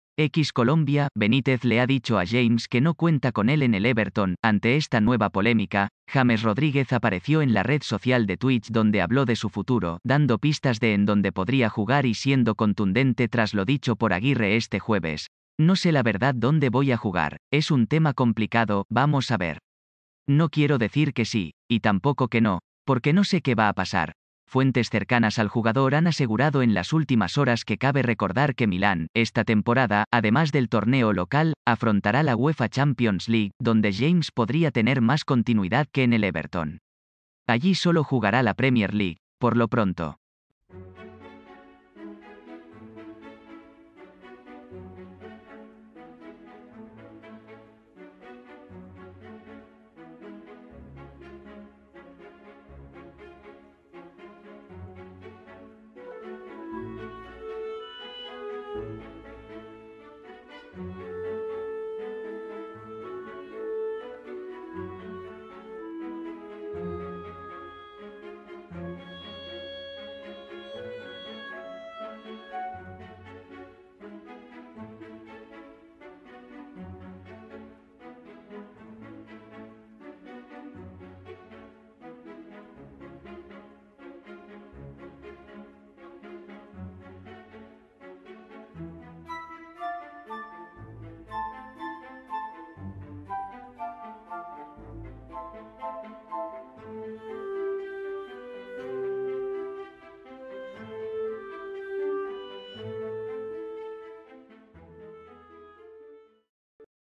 “Uno tiene que estar donde lo quieren a uno”: James Rodríguez, contundente tras su posible salida d
❌🇨🇴 "BENÍTEZ le ha dicho a JAMES que NO CUENTA con él en el EVERTON"Ante esta nueva polémica, James Rodríguez apareció en la red social de Twitch donde habló de su futuro, dando pistas de en donde podría jugar y siendo contundente tras lo dicho por Aguirre este jueves: “No sé la verdad dónde [...]